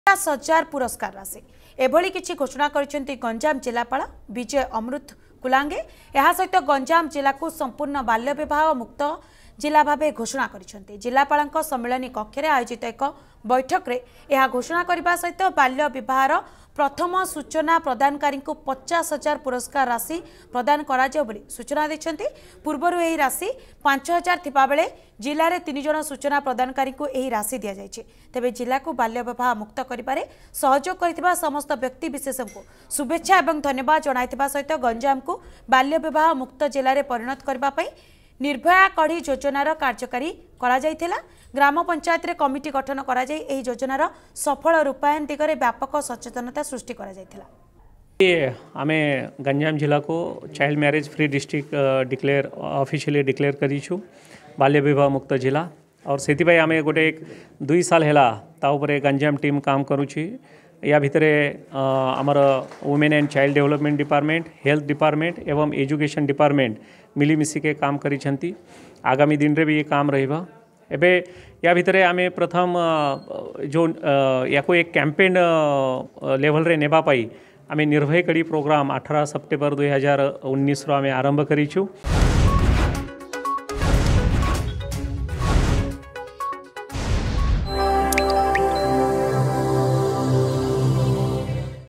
ଗଞ୍ଜାମ ଜିଲ୍ଲାକୁ ସମ୍ପୂର୍ଣ୍ଣ ବାଲ୍ୟ ବିବାହ ମୁକ୍ତ ଜିଲ୍ଲା ଭାବେ ଘୋଷଣା
ଏଣିକି ଗଞ୍ଜାମରେ ବାଲ୍ୟ ବିବାହର ପ୍ରଥମ ସୂଚନା ପ୍ରଦାନକାରୀଙ୍କୁ ମିଳିବ 50 ହଜାର ପୁରସ୍କାର ରାଶି । ଏଭଳି କିଛି ଘୋଷଣା କରିଛନ୍ତି ଗଞ୍ଜାମ ଜିଲ୍ଲାପାଳ ବିଜୟ ଅମୃତ କୂଲାଙ୍ଗେ । ଏହାସହ ଗଞ୍ଜାମ ଜିଲ୍ଲାକୁ ସମ୍ପୂର୍ଣ୍ଣ ବାଲ୍ୟ ବିବାହ ମୁକ୍ତ ଜିଲ୍ଲା ଭାବେ ଘୋଷଣା କରିଛନ୍ତି ।